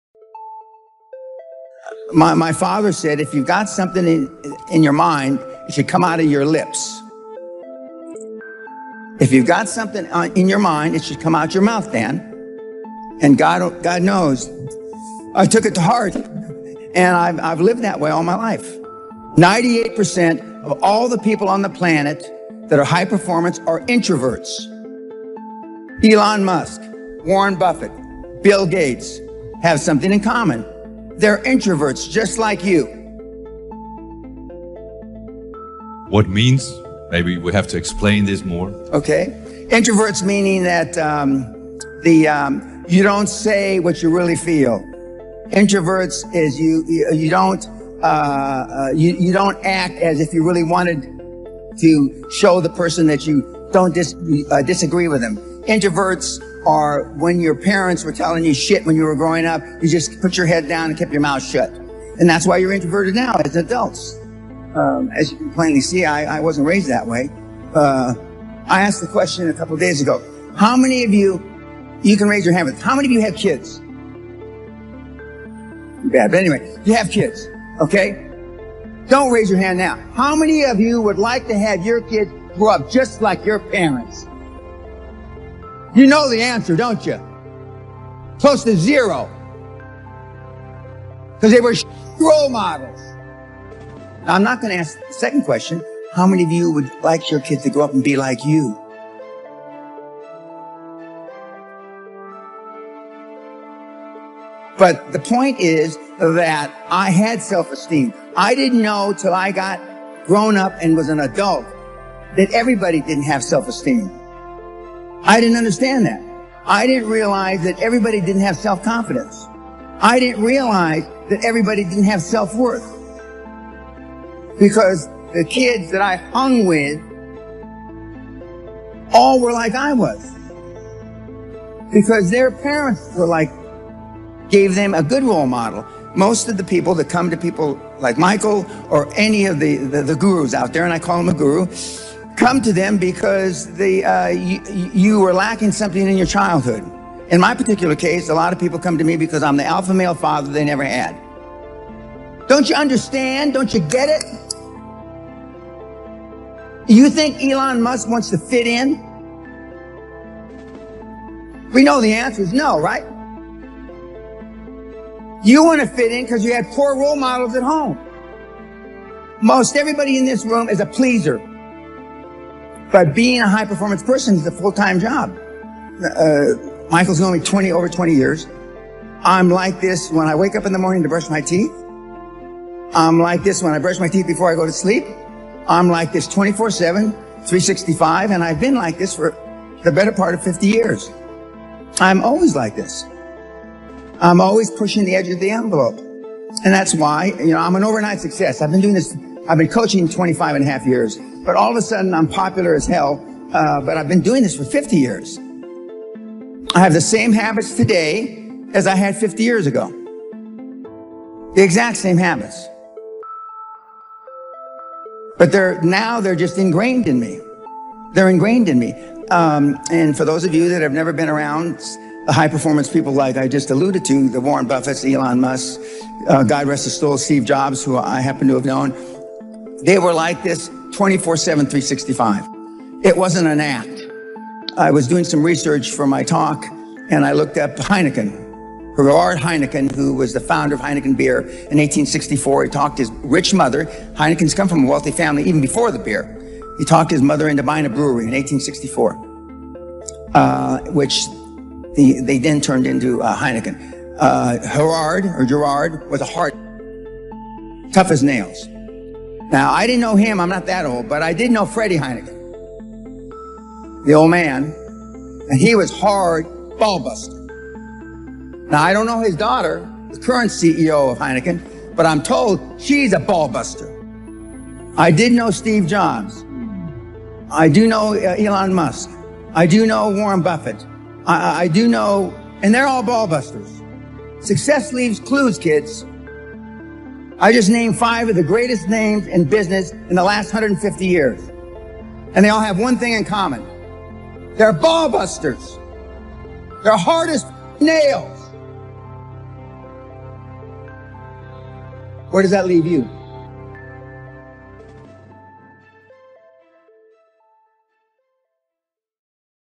WEALTHY MINDSET - Best Motivation Video 2022 - Dan Pena
DESCRIPTION

“More people should learn to tell their dollars where to go instead of asking them where they went."

►Speaker:Dan Peña

Dan Peña, also known as the 50 billion dollar and trillion dollar man,  is an American businessman and multi-millionaire. He is the founder of Quantum Leap Advantage Methodology and chairman of The Guthrie Group, an investment consortium.